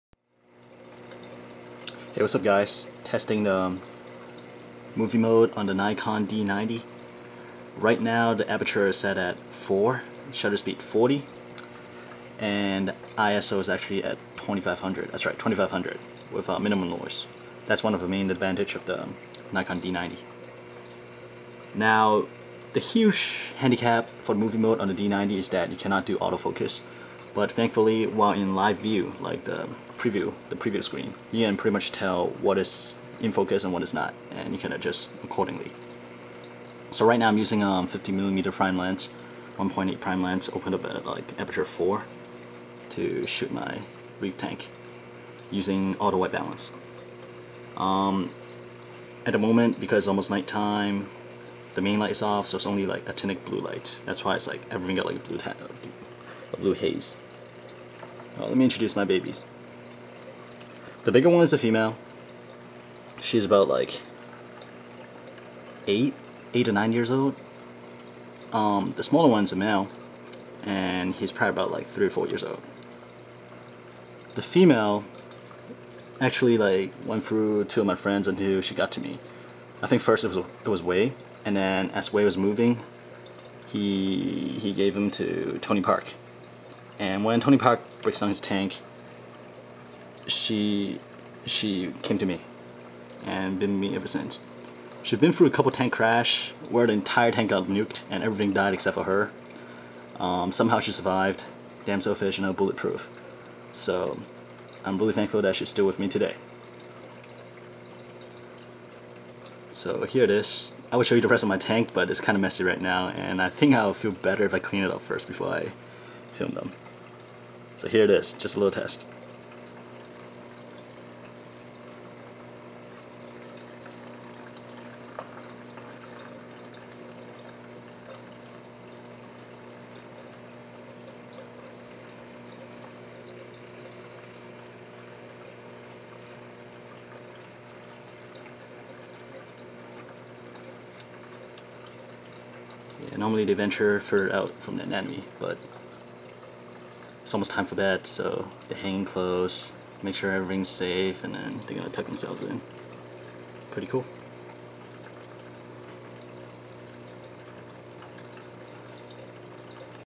Nikon D90 video test - Clown Fish and Anemone (HD)
Testing the Nikon D90 HD video function, using a 50mm f1.8 prime lens.